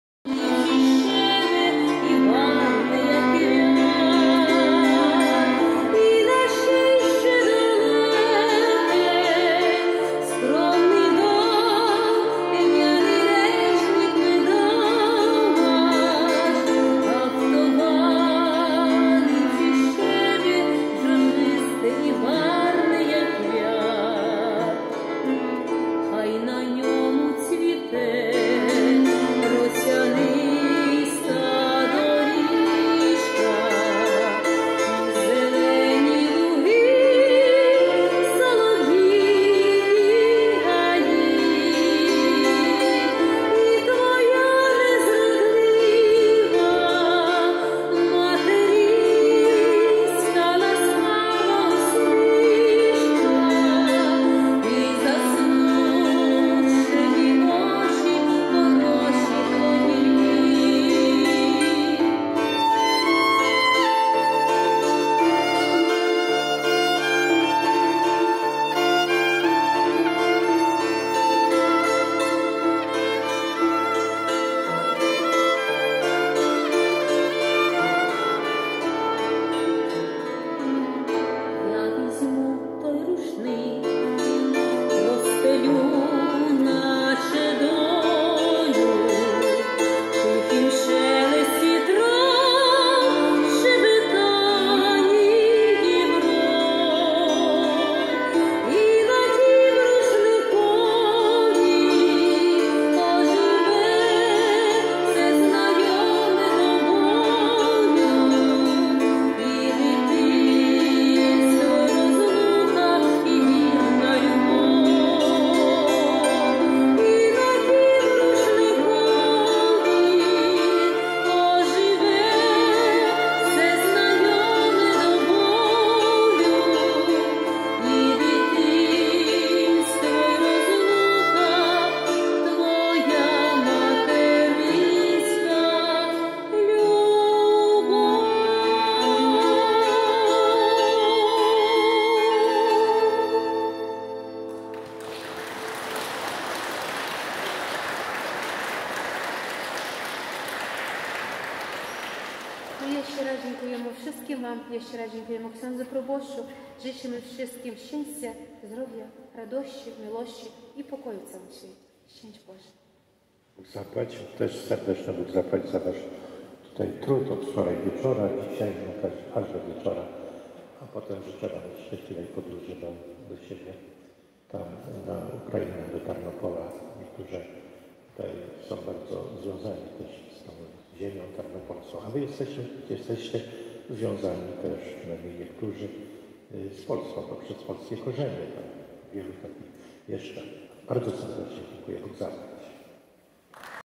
Zespół śpiewaczy „Zbrucz” z Tarnopola w naszej parafii.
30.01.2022 w parafii NMP Królowej Polski w Głogowie liturgie ubogacał zespół śpiewaczy „Zbrucz” z Tarnopola.